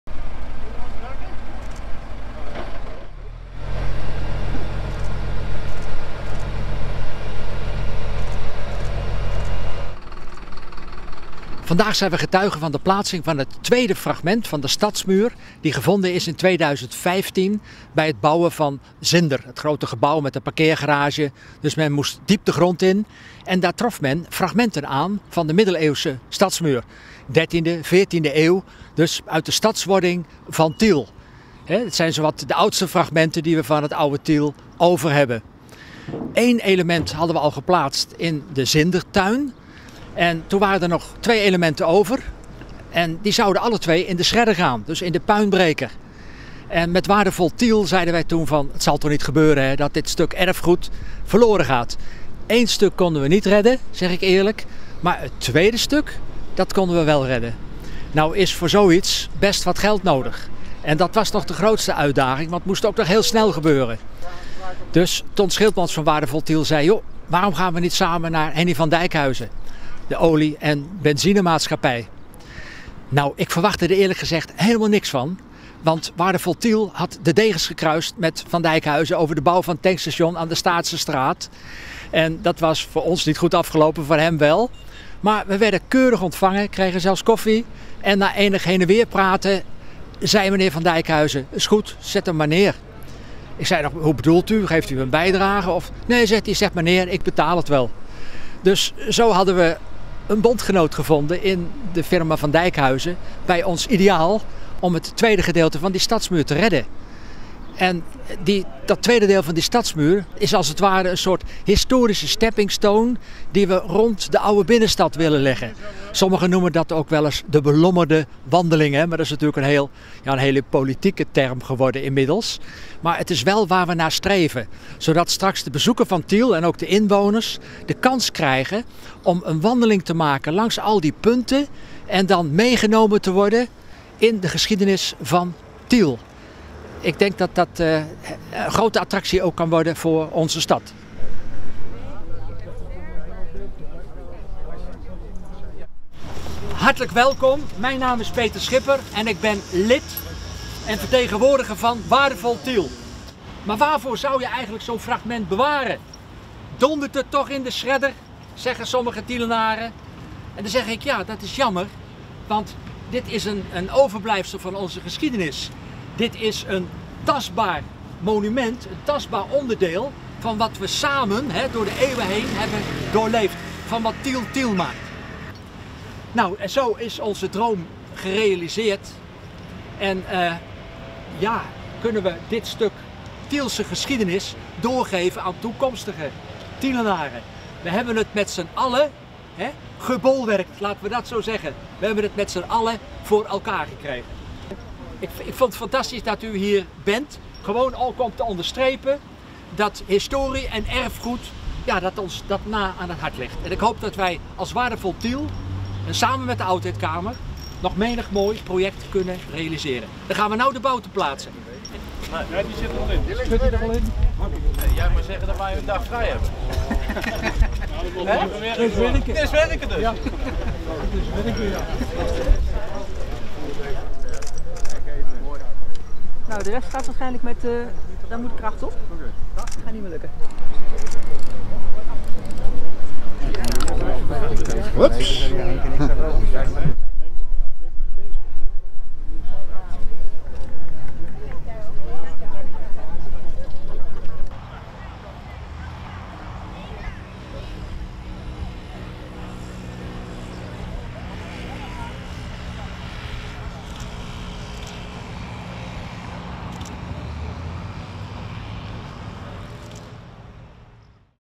20210716 Tiel Stadsmuur Westluidensepoort bij Paardenwater geplaatst
Tiel, 16 juli 2021
Aan het Paardenwater is het tweede stuk van de Tielse stadsmuur geplaatst. Een flinke brok middeleeuws muurwerk dat in 2014 werd aangetroffen bij de graafwerkzaamheden voor de bouw van Zinder en de parkeergarage aan de Westluidense Poort. Initiatiefnemer Peter Schipper van Waardevol Tiel tijdens een bijeenkomst na afloop samen met andere betrokkenen.Henny van Dijkhuizen maakte het financieel mogelijk. (wilde liever niet op de foto)
Video: Foto Jan Bouwhuis.